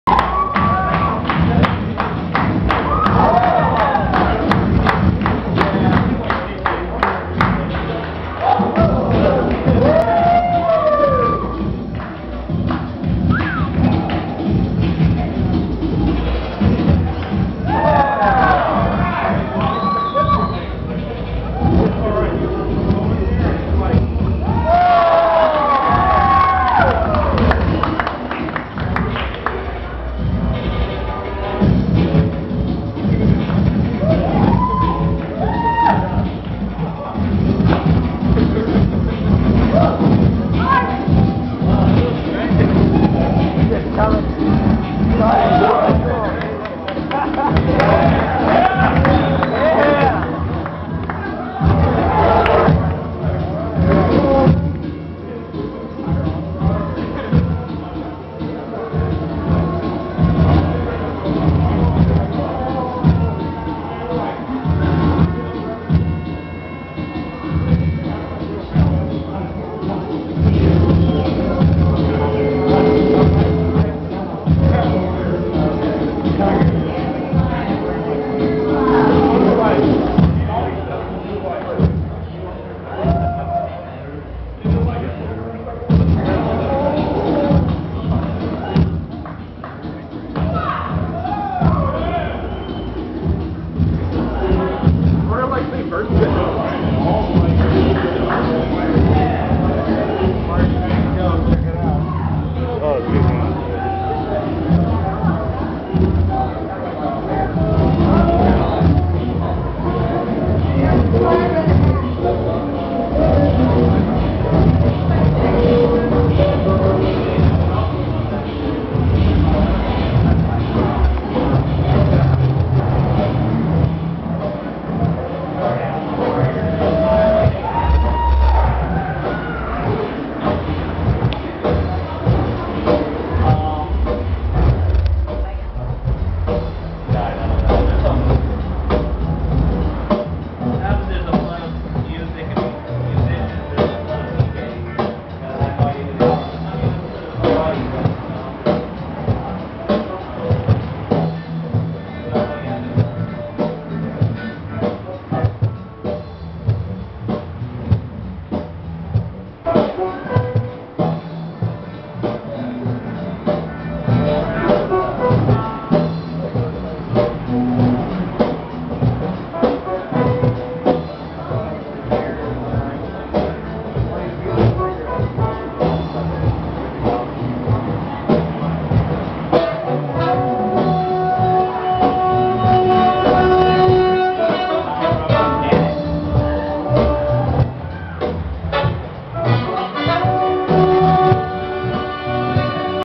Kid feeling the beats and doing flips at the Freak Show Downtown
and rocking the crowd!